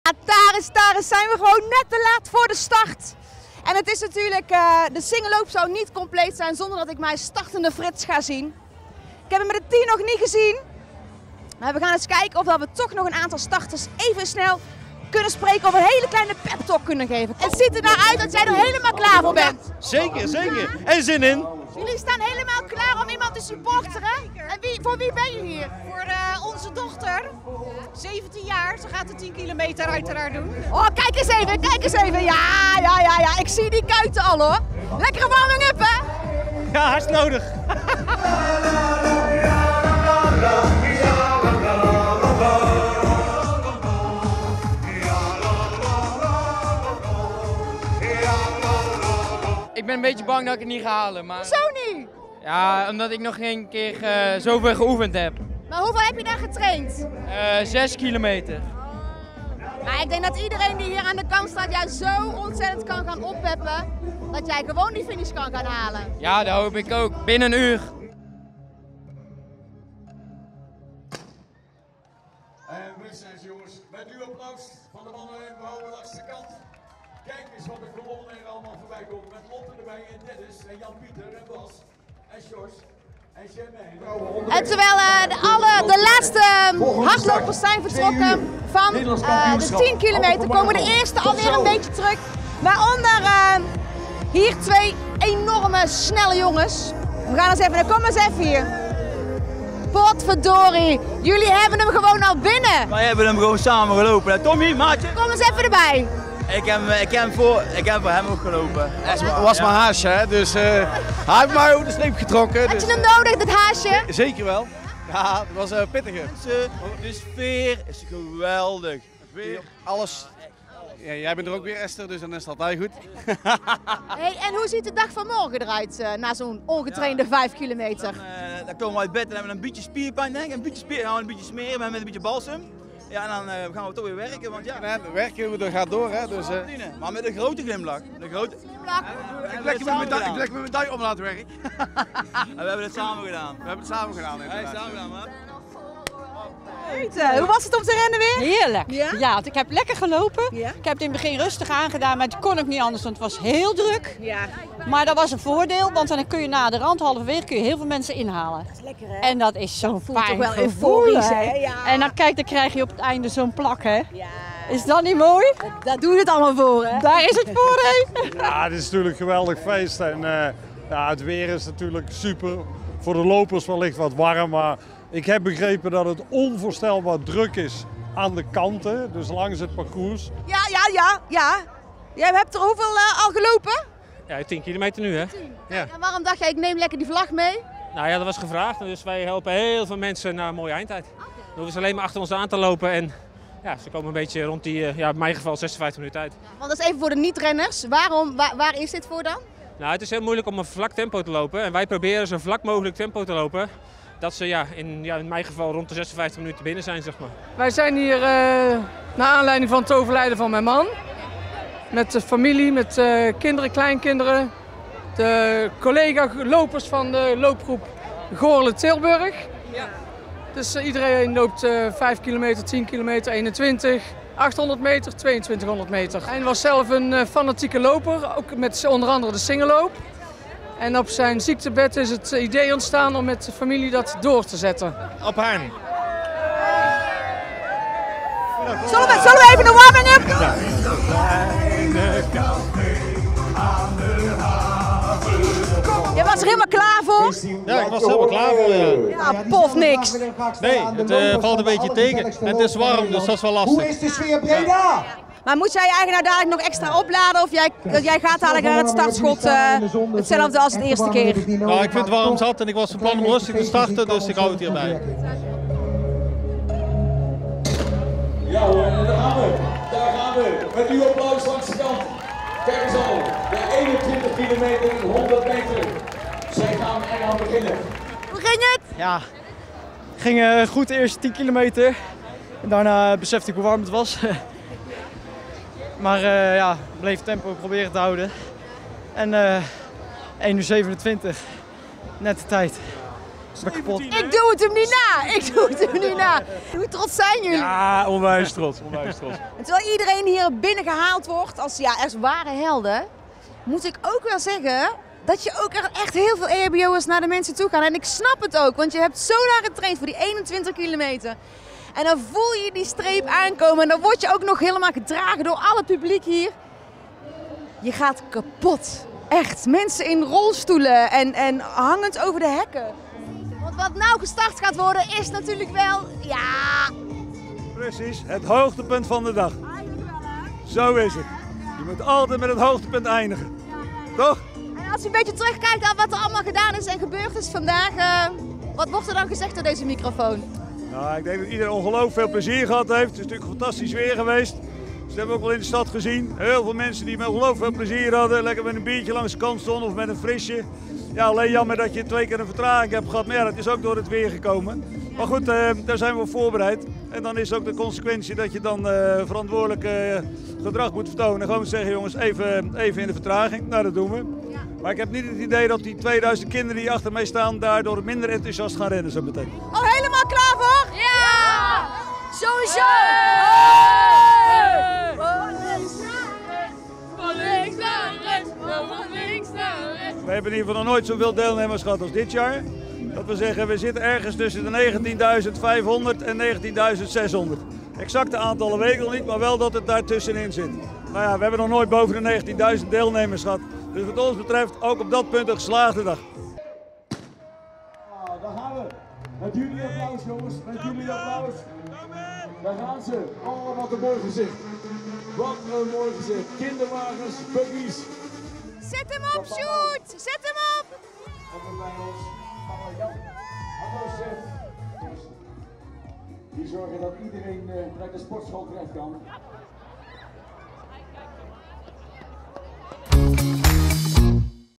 Sfeer proeven tijdens de Bredase Singelloop 2023 | Breda in Beeld
Afgelopen zondag vormde de Bredase binnenstad traditiegetrouw het decor voor de Singelloop. Het grootste hardloopevenement uit de regio, met afstanden van vijf en tien kilometer en de halve marathon, trok niet alleen veel hardloopfanaten maar natuurlijk ook veel bezoekers. Esther ging op pad en proefde de sfeer in de stad.